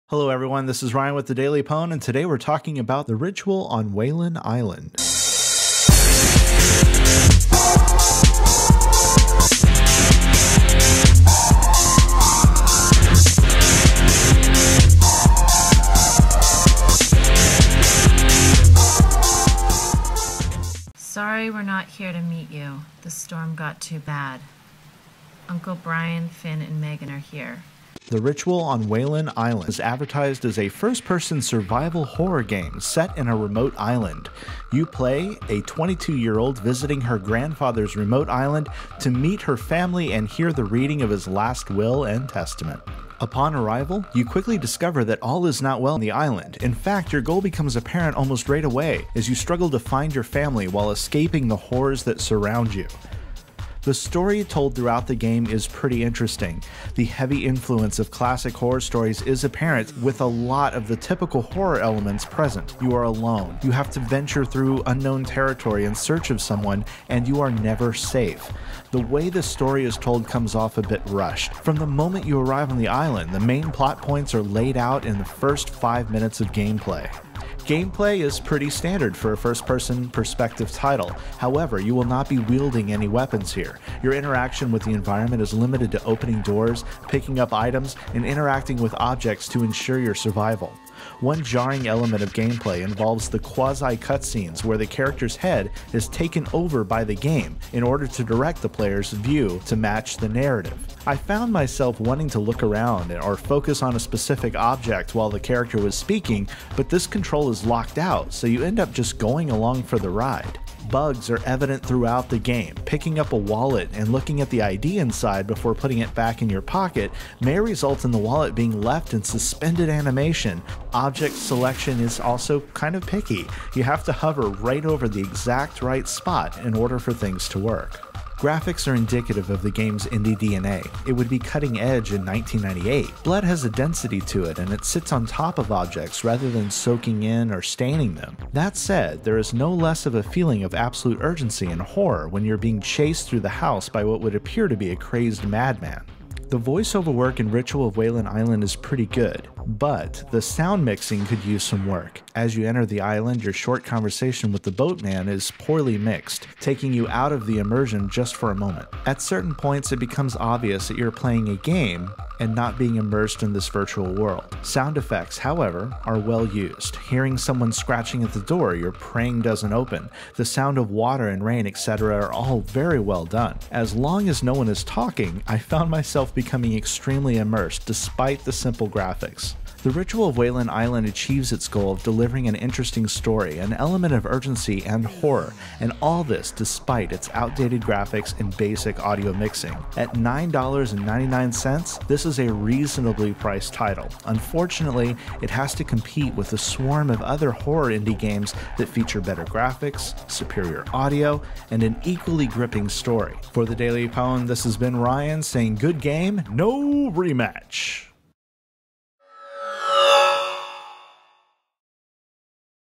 The Ritual on Weylyn Island Review
The Ritual on Weylyn Island is advertised as a first-person survival horror game set in a remote island. You play Moira Weylyn, a 22-year-old visiting her grandfather’s remote island to meet with her family and hear the reading of his last will and testament.